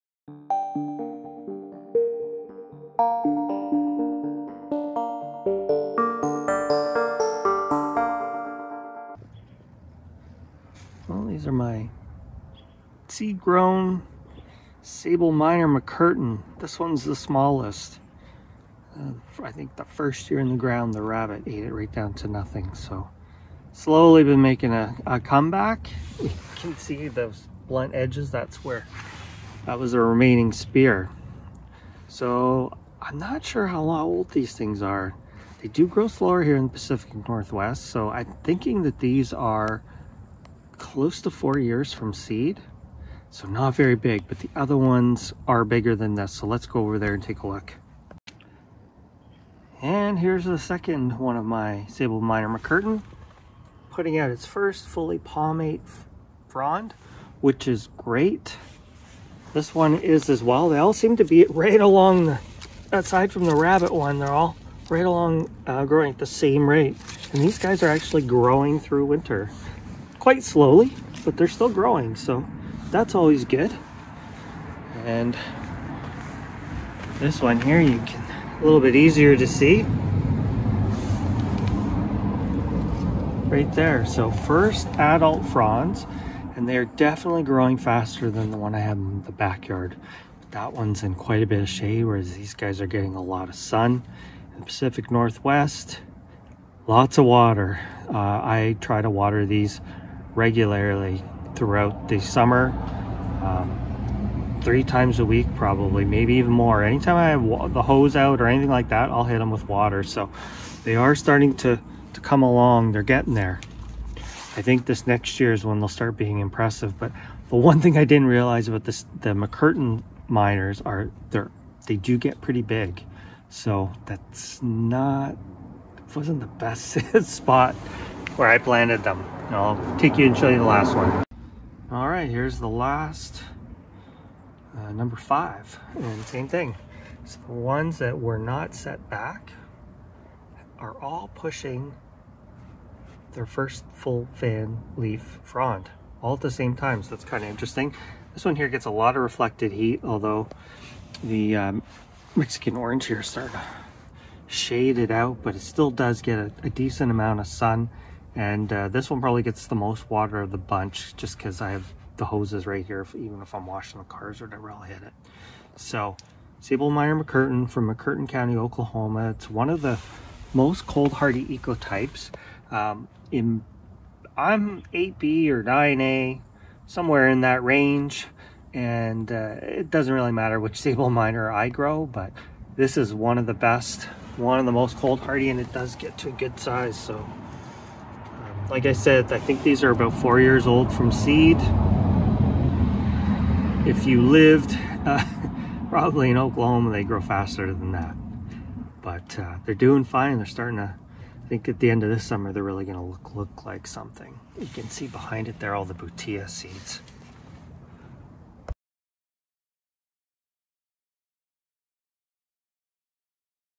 Sabal Minor "McCurtain" - Growth rate
These are my own seed grown Sabal minor from McCurtain county Oklahoma.  They are considered to be one of the hardiest ecotypes of Sabal minor if not the hardiest.  Many people are having success growing these in zone 6.